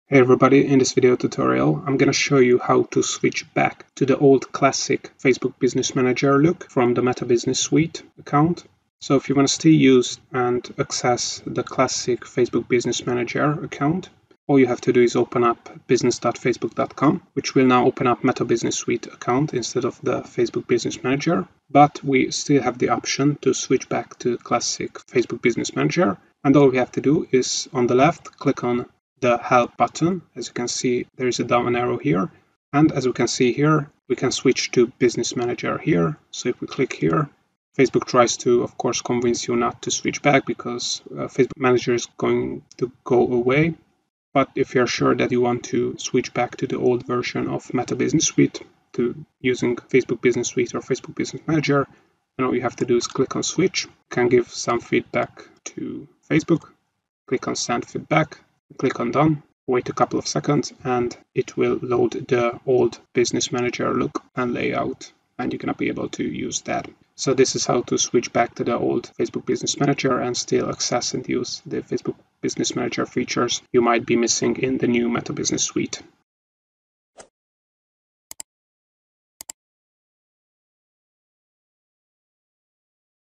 How To Switch Back To Facebook Business Manager? [in 2025] (Access Facebook Business Manager)
. - Learn how to switch back to access and use the old Facebook Business Manager if you don't like the new Meta Business Suite look.

In this video tutorial, we are going to show you to switch back to the classic Facebook Business Manager from the Facebook/Meta Business Suite.

☑️ Complete tutorial on how to switch back to classic Facebook Business Manager